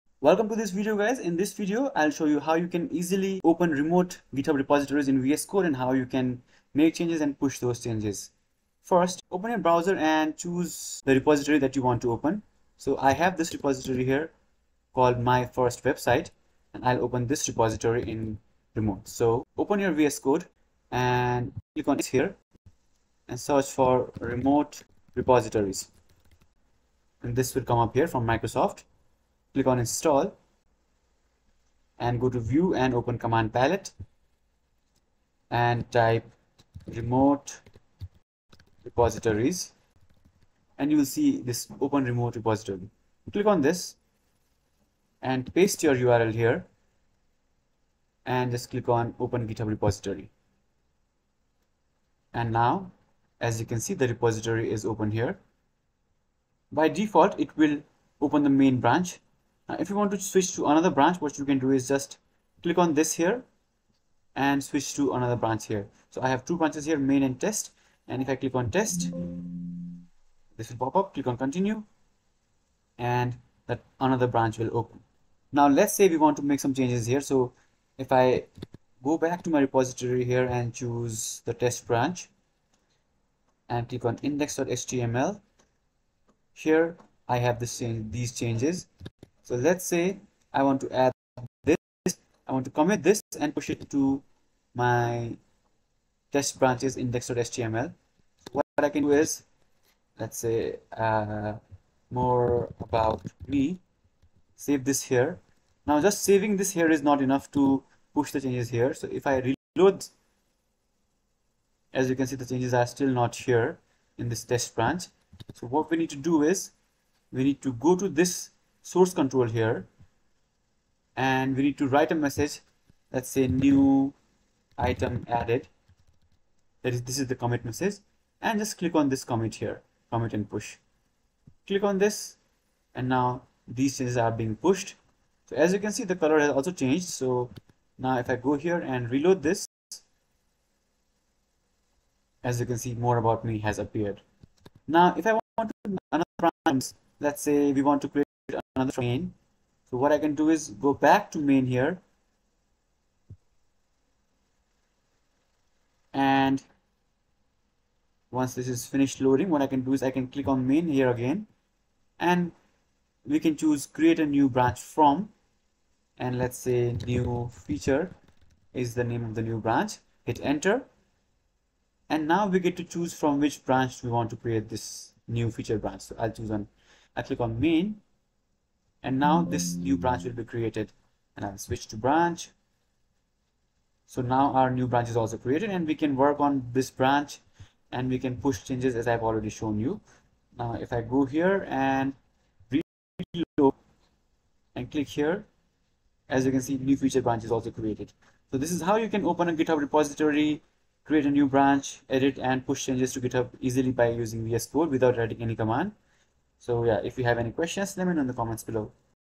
How to Open Github Repositories in Visual Studio Code (2024)
In this video, I'll show you how you can open remote github repository in github easily. You can easily open github repositories in vscode without having to download or clone the repository manually. We will use the remote repositories extension provided by microsoft to do this.

Steps to Open Github Repository in VSCode:
1. Open the github repository in your browser
2. Copy the repository URL
3. Open Visual Studio Code (vscode)
4. Install the remote repositories extension
5. Open command palate
6. Enter Open remote repository and paste your url

By the end of this video, you'll see how you can easily open github repository in visual studio code, and how you can commit and push changes and create new branch from vscode itself.